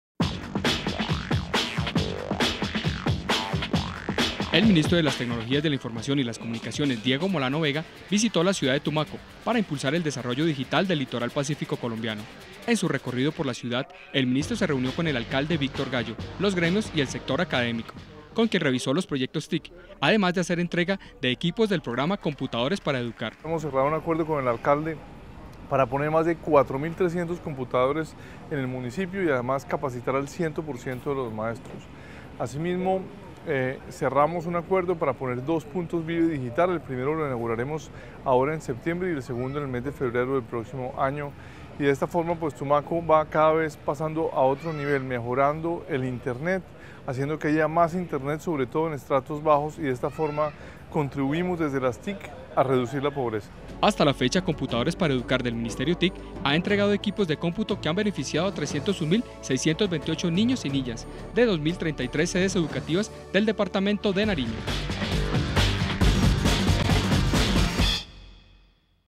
Tumaco en busca del desarrollo a través de las TIC
El Ministro TIC Diego Molano Vega llegó al puerto nariñense para impulsar el desarrollo digital del litoral pacífico colombiano. El Gobierno reiteró su convencimiento de que la prosperidad está íntimamente relacionada con la pronta implementación del uso de las tecnologías de la información y las comunicaciones en todas las poblaciones de Colombia.